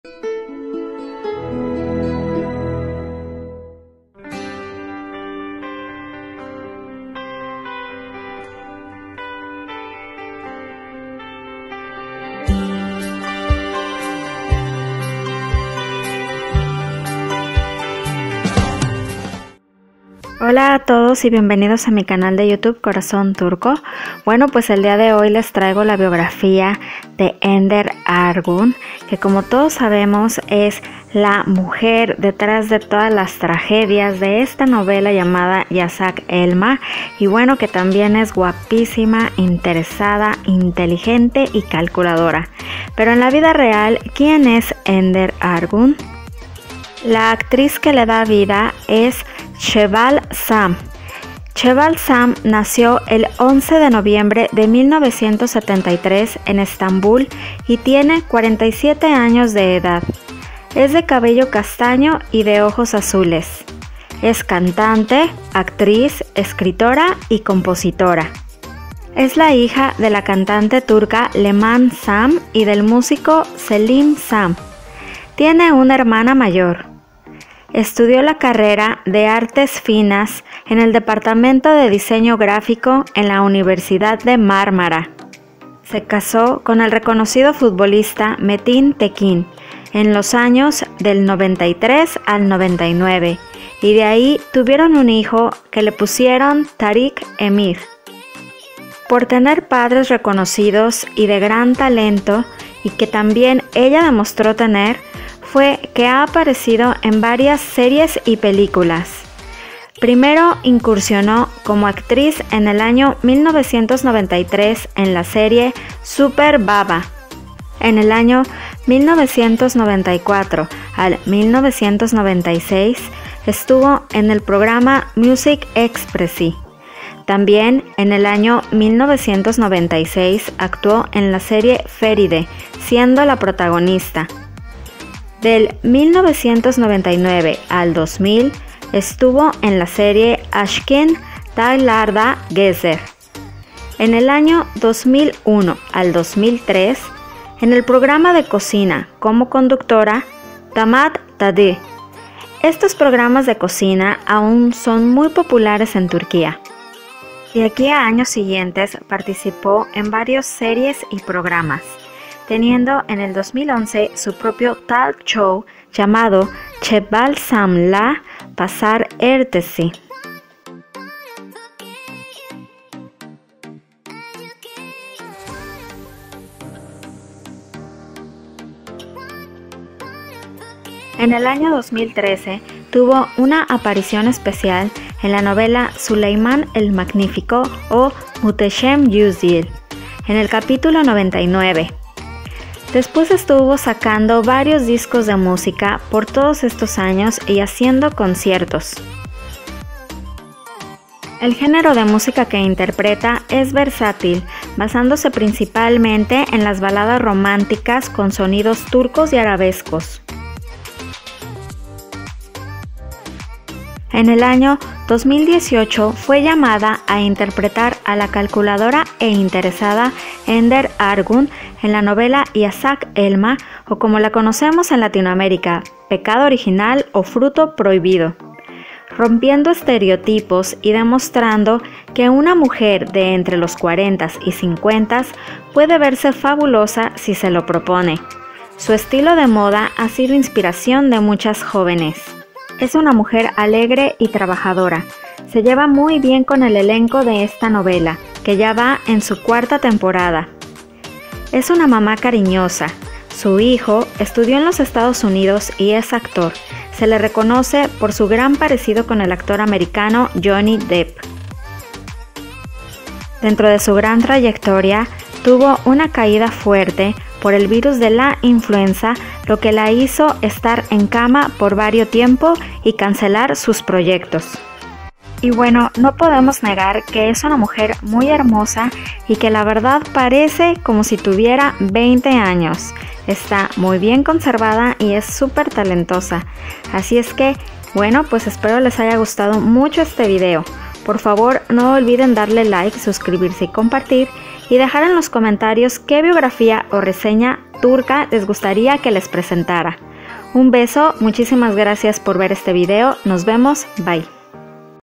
QUİEN ES? ENDER ARGUN/ PECADO ORİGİNAL/ YASAK ELMA NOVELA TURCA
Amig@s hoy les traigo la biografia de la actriz turca que interpreta a la codiciada Ender de la novela pecado original o fruto prohibido para que la conozcan y sepan de la trayectoria que tiene aqui en Turquia.

Espero les haya gustado mucho este video
Soy Lorena Mexicana viviendo en Turquia, casada con un Turco ya desde hace 3 anıos.